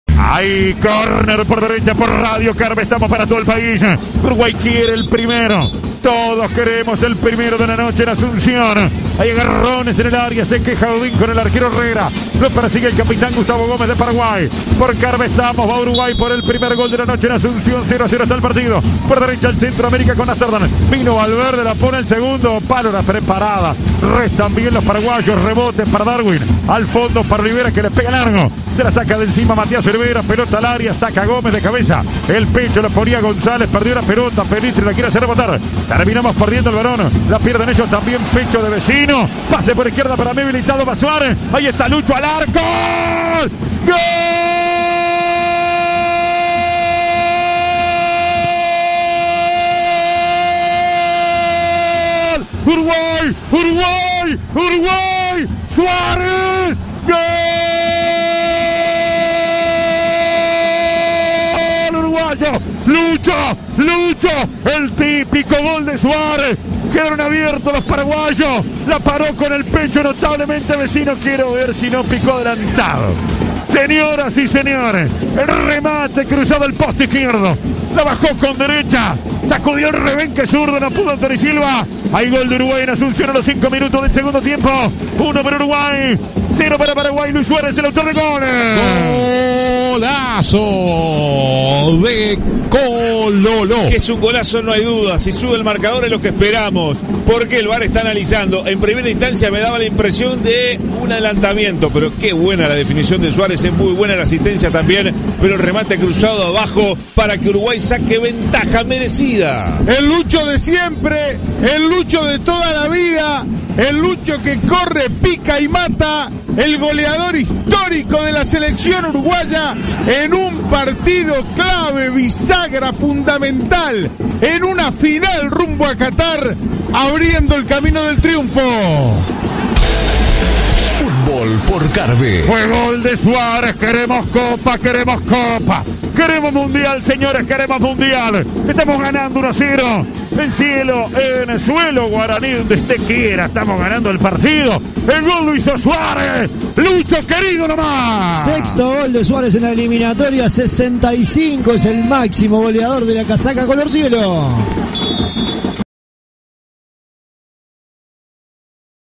Paraguay 0 Uruguay 1 - Eliminatorias Qatar 2022 (por Gabriel Regueira)
El gol de Suárez, por Gabriel Regueira junto a Martín Díaz y Pablo Karslian para CX 16 Radio Carve 850 AM.